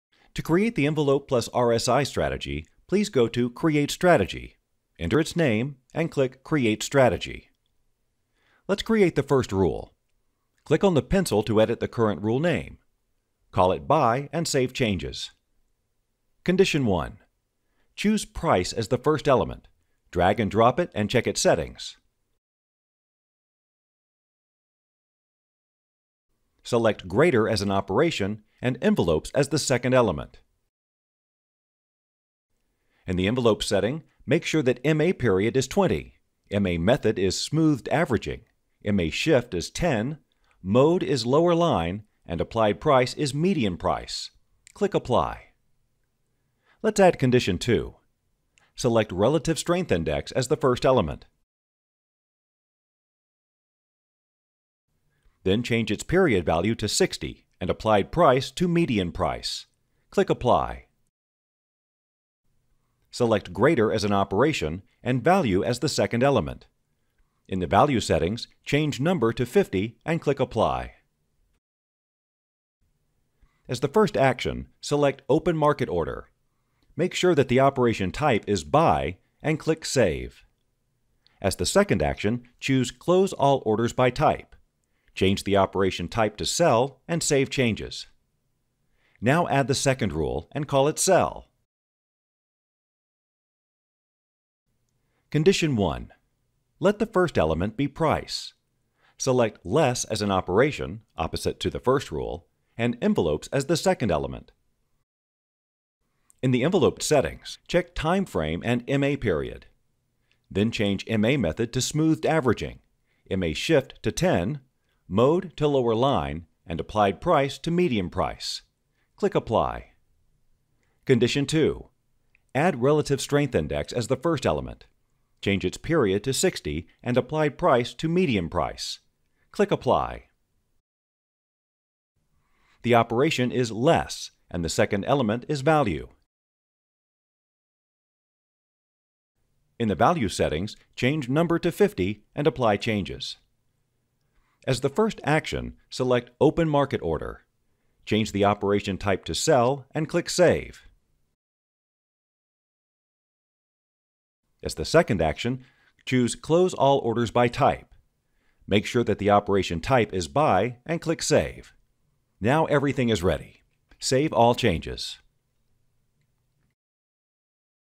Building the Envelopes+RSI strategy in VSB [Step-by-step guide]
If you saw our previous videos, you already know that Visual Strategy Builder is a simple program that serves as a constructor: any user can build a custom strategy (Expert Advisor) while just moving the appropriate elements and editing its settings.

In this tutorial, we will teach you – with the Envelopes+RSI strategy as an example – how to create an EA using the tools available in Visual Strategy Builder (Elements, Operations, Actions) and, what’s the most important, how to make them work!

You’ll get acquainted with a profitable technical strategy consisting of two famous indicators, Envelopes and Relative Strength Index (RSI), allowing you to detect and profit from strong trending movements. The Envelopes are used here to seek trends while the RSI indicator helps to pick the direction for entry.

Watch the video and try to create the strategy in VSB by yourself!